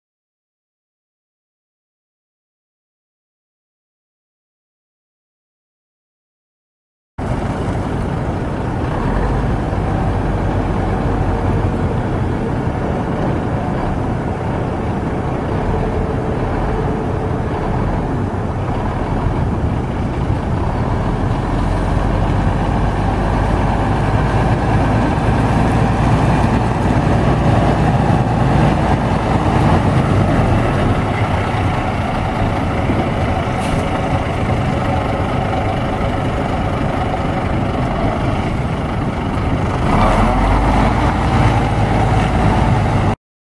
1992 Mack CH613 semi truck for sale | sold at auction January 21, 2014
1992 Mack CH613 semi truck
111,233 miles on odometer
44,888 hours on meter
Mack E7 12.0L L6 diesel engine
Eaton Fuller seven speed manual transmission
AC and heat
AM/FM/cassette
Quarter fenders
Spring suspension
Air brakes
295/75R22.5 tires
Interior/exterior cosmetic damage
Unit # 1347
1M2AA12YXNW018533